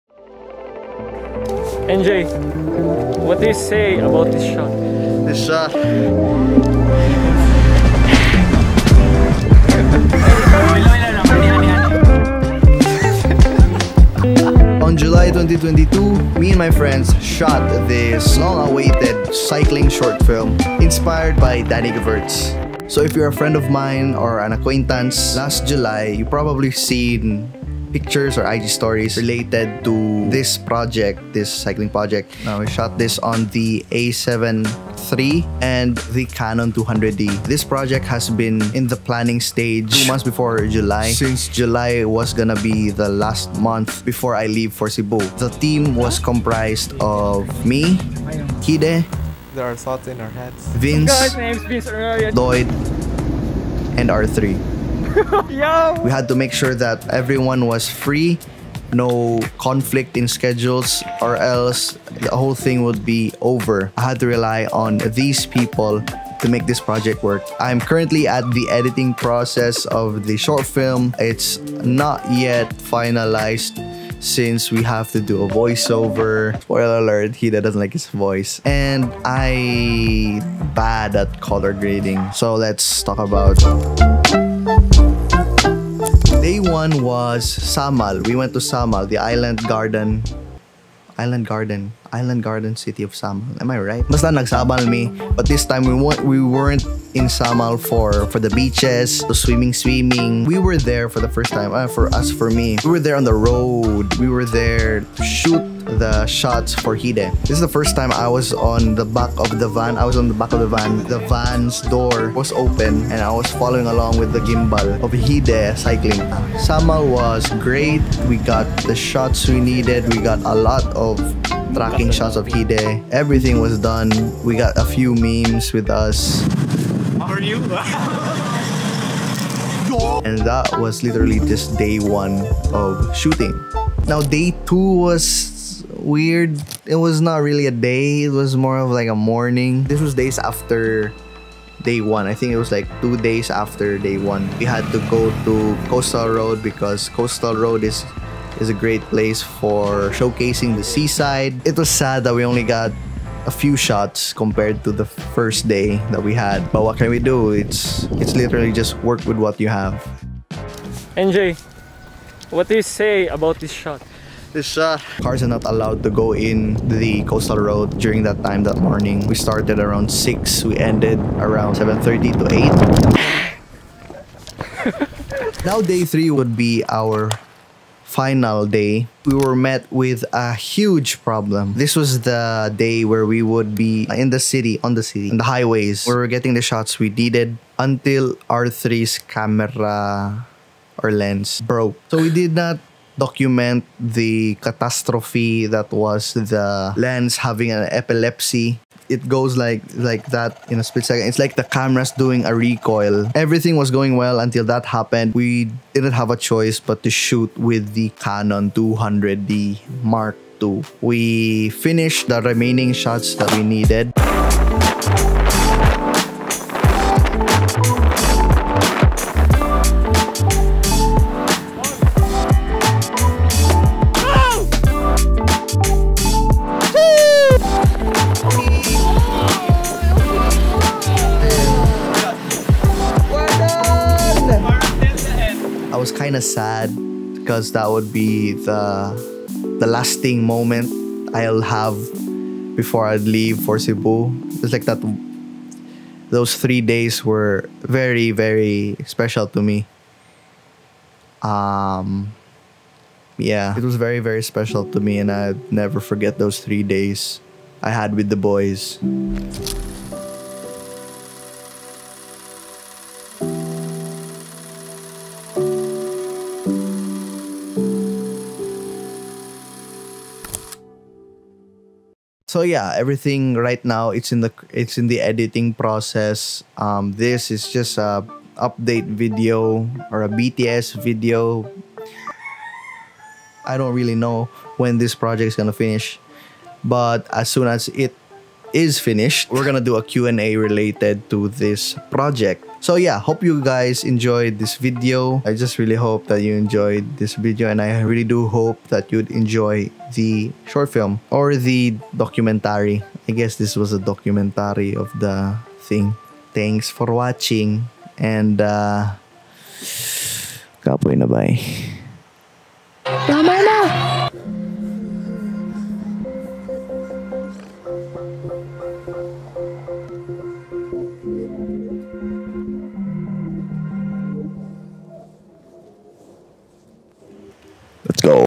Shooting a Cycling Short Film in Three Days
We shot a short film/documentary regarding cycling and here's a short behind the scenes video on how it went :)

Super stoked to see how this project will turn out after its finalization and we really hope all of you will tune in and enjoy this project as much as we had fun making it! I'd like to give a huge thanks to the guys making this passion project possible.

Timestamps:
0:00 - Intro
1:22 - Day 1
2:13 - Day 2
2:55 - Day 3
3:54 - Maoy Things
4:32 - Outro